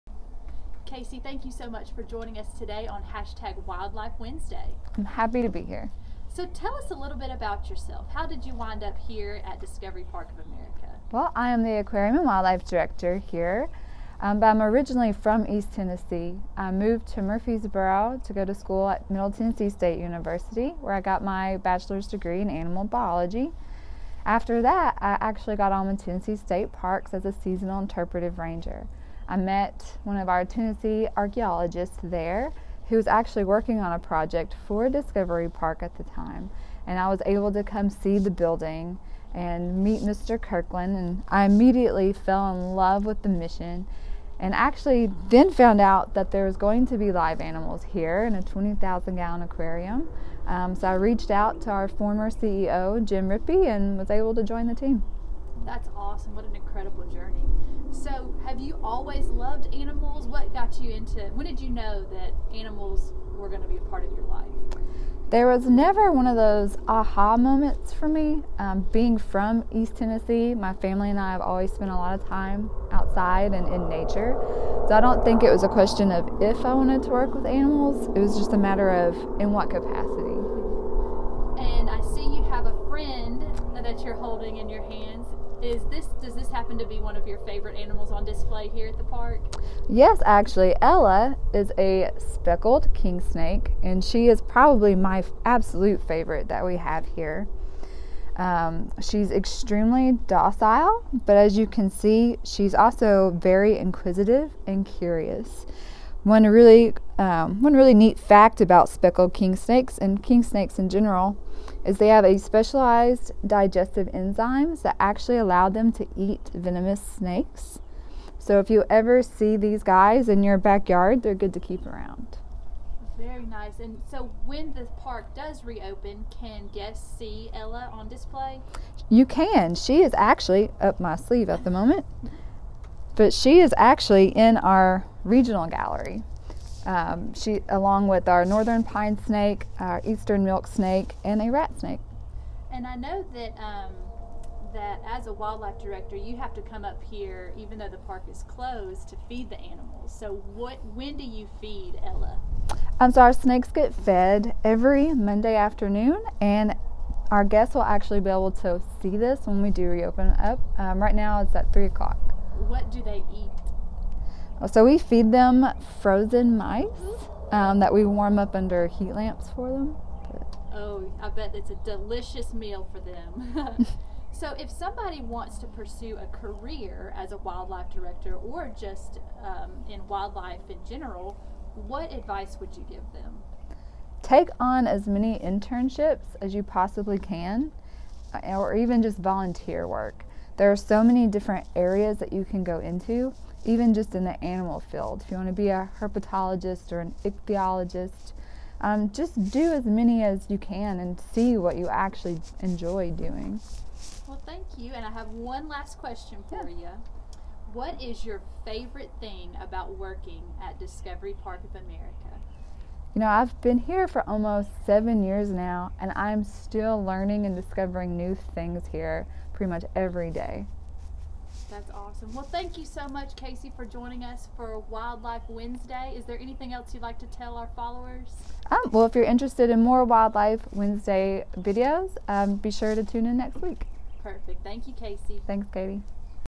WildlifeWednesday: Speckled King Snake
Kacey King, aquarium and wildlife director, talks about how she got involved with wildlife and her journey to Discovery Park of America. She even brought her friend Ella🐍 along for the interview.